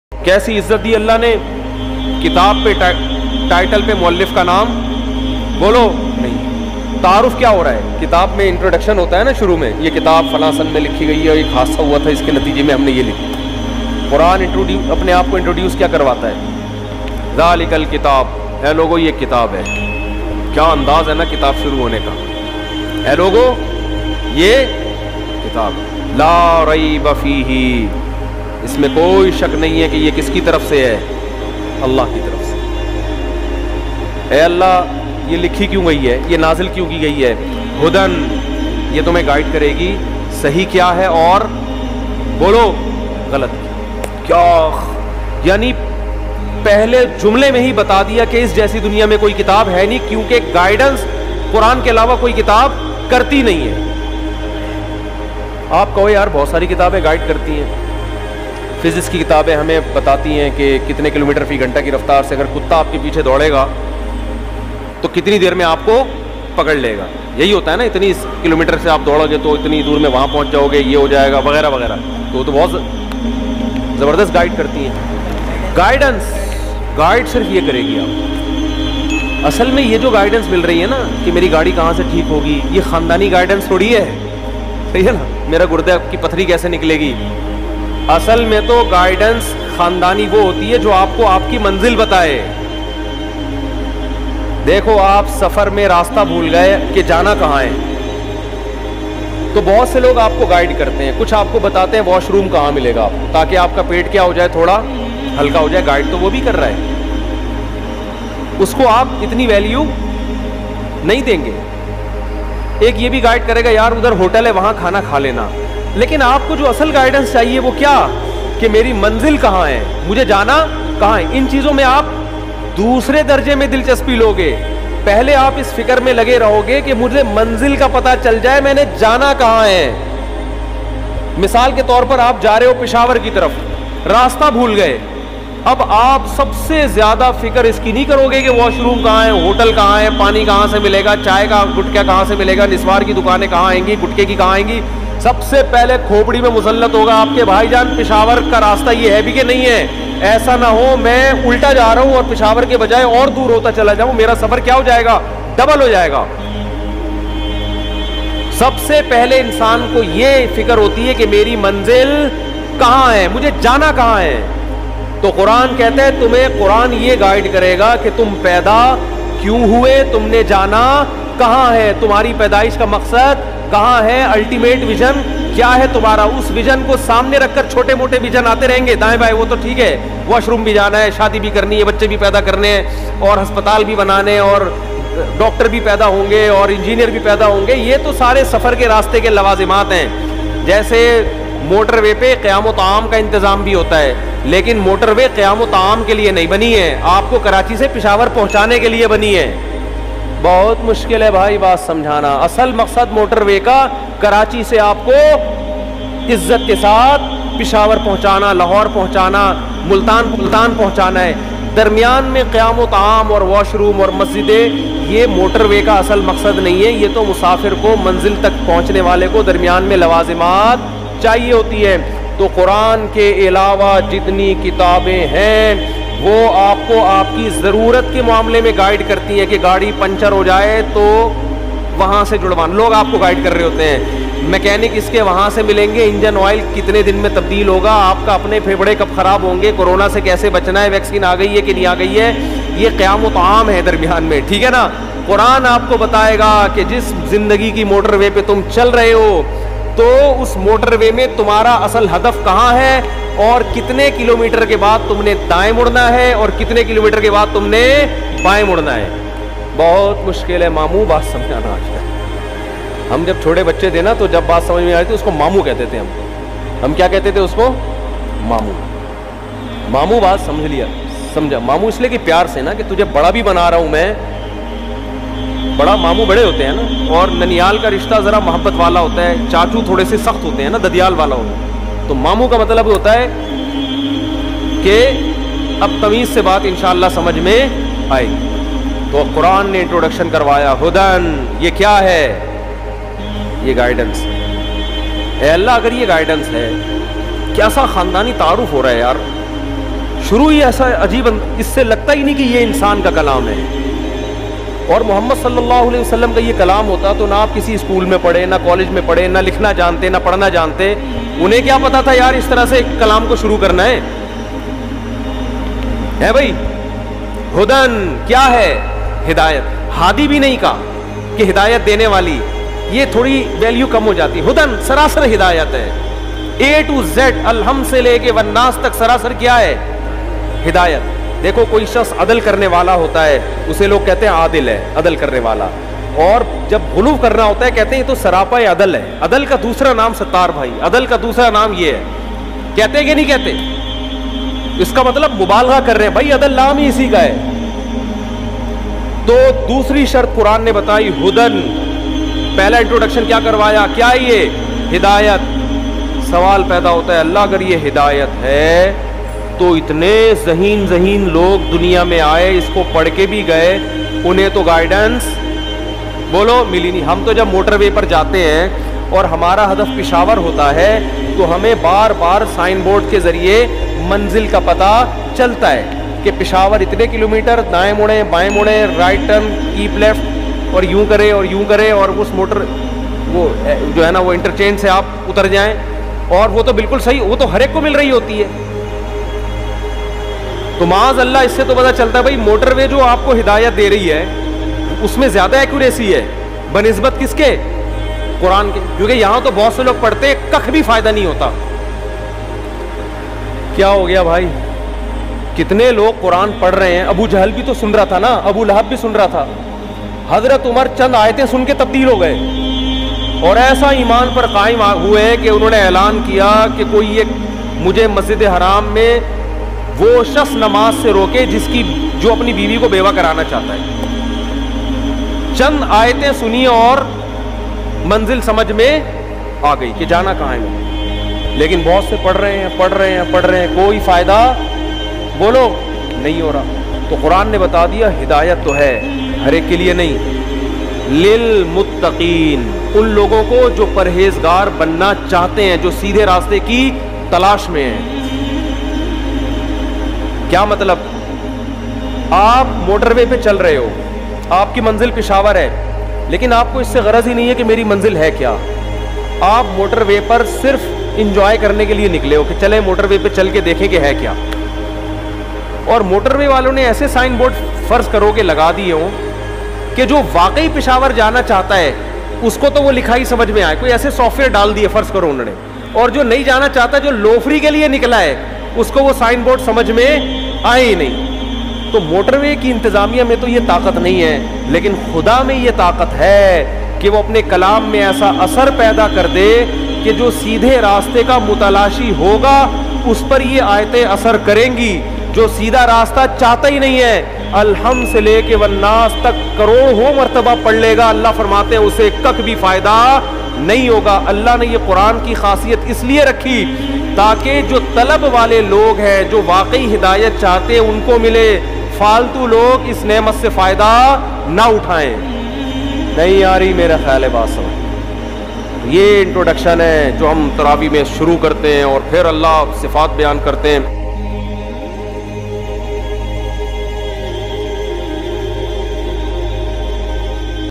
Quran Agar Hidayat Hai Toh Duniya Ke Zaheen Lago Isse Hidayat Kyu Nahi Mili | Mufti Tariq Masood
▪️Credit : Mufti Tariq Masood Speeches